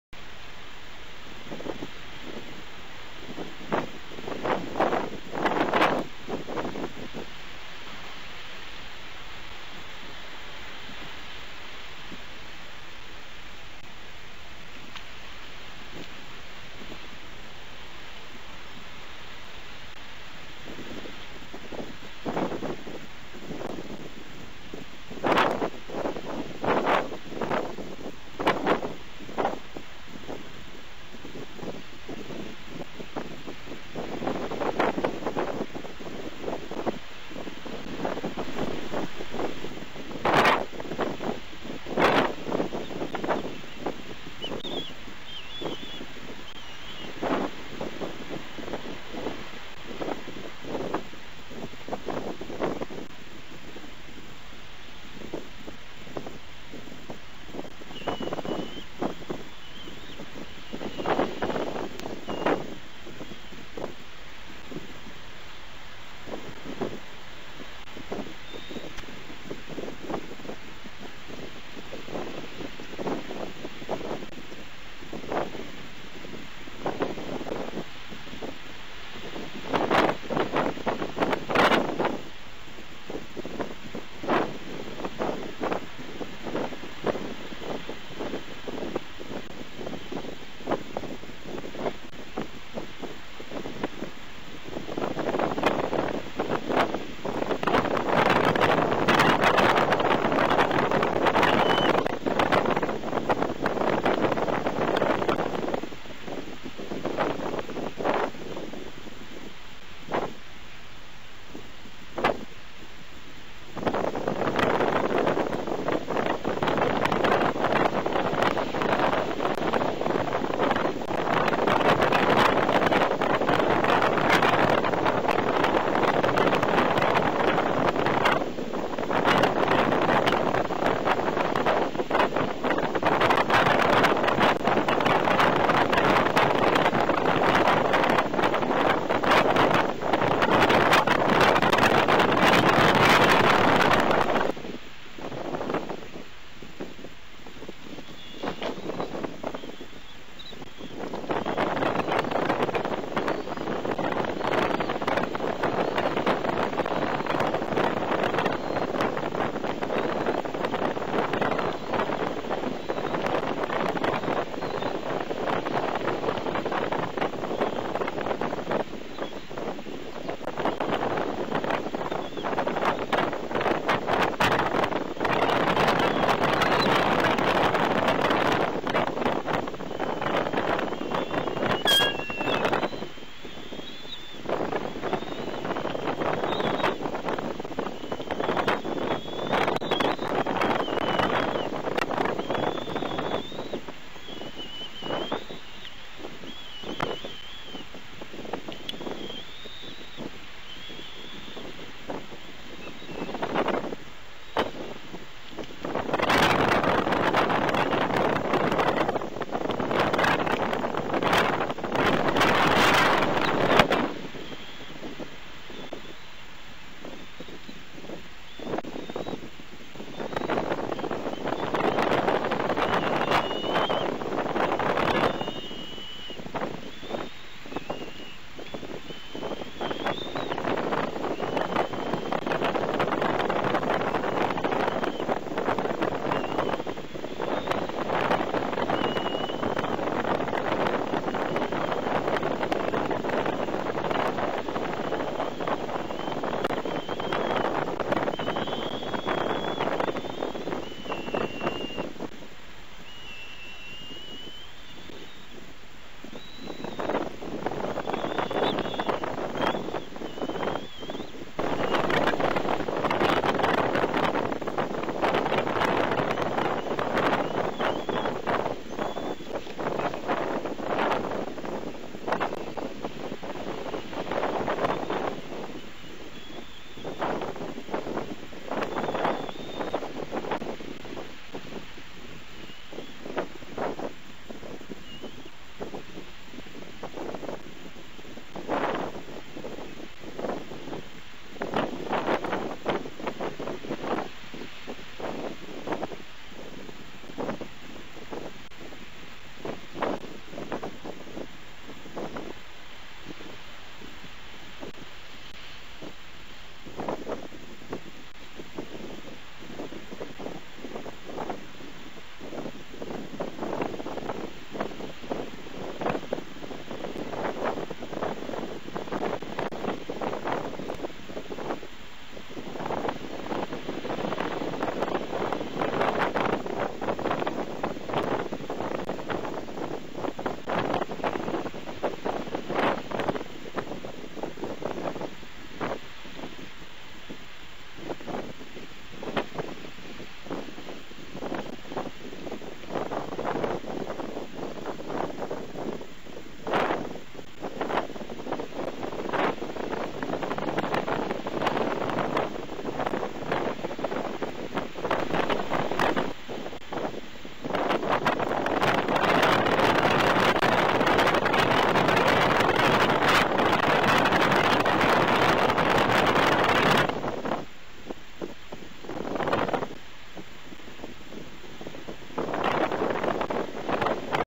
Wingercizes West End Style!.wmv
The eaglets at the WE Nest are testing their wings. We also see how the cam lens got 'fowled' up too!